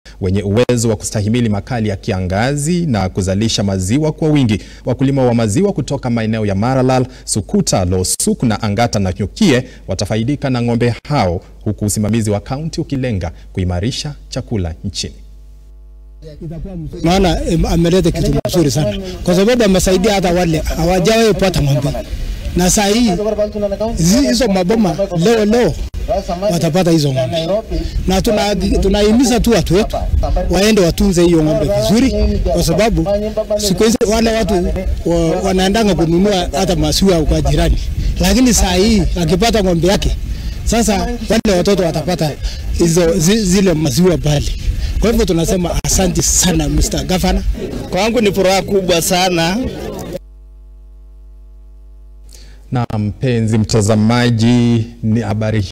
Zaidi Ya Ngombe 900 Zatolewa Kwa Wafugaji Samburu
Kilimo Cha Maziwa Nchini

Katika Azma Ya Kuinua Kilimo Cha Maziwa Nchini, Serikali Ya Kaunti Ya Samburu Imewafaidi Wafugaji Na Aina Mpya Ya Ngombe Wajulikanao Kama Sahiwal Heifer Wenye Uwezo Wa Kustahili Makali Ya Kiangazi Na Kuzalisha Maziwa Kwa Wengi.